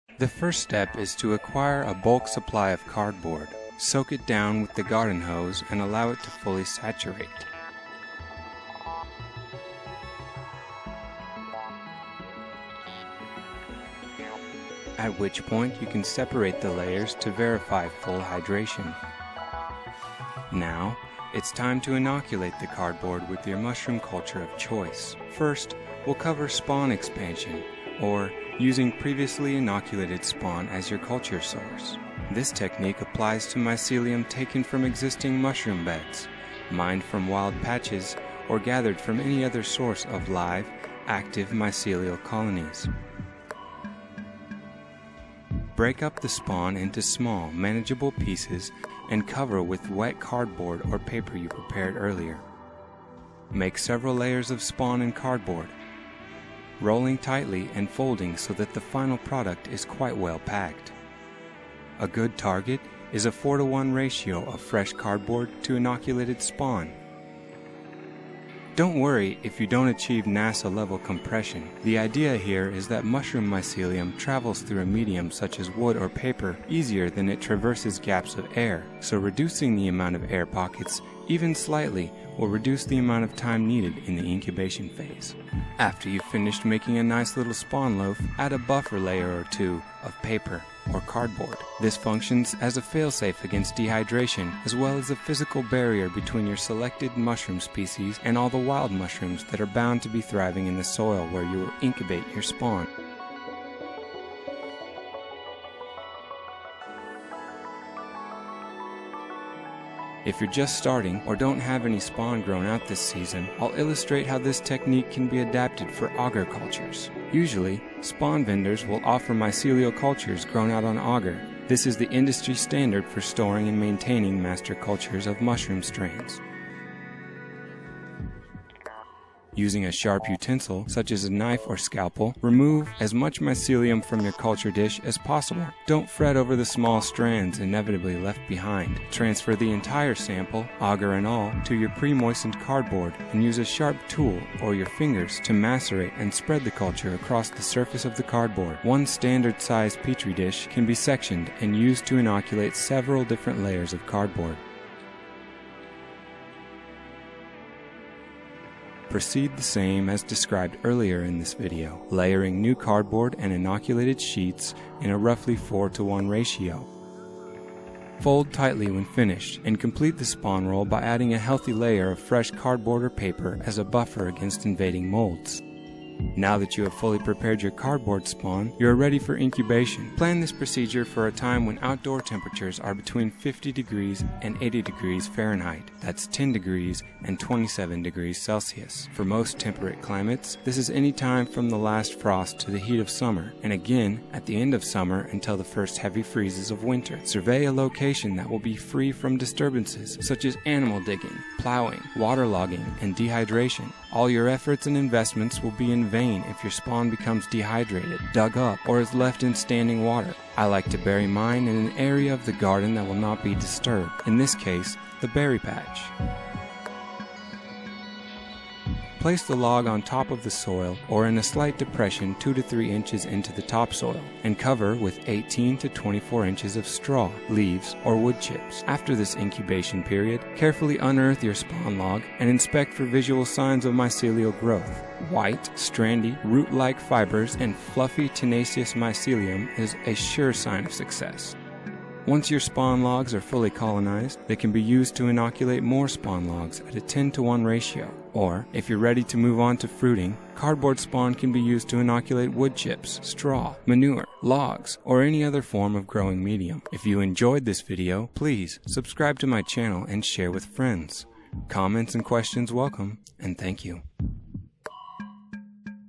Growing Edible Mushrooms at Home - Cardboard Spawn Tutorial
An in-depth instructional video detailing how anyone can start growing gourmet and medicinal mushrooms at home, without spending a cent.   Recycle your cardboard waste, creating spawn cultures that can be used to inoculate long-lasting outdoor mushroom beds.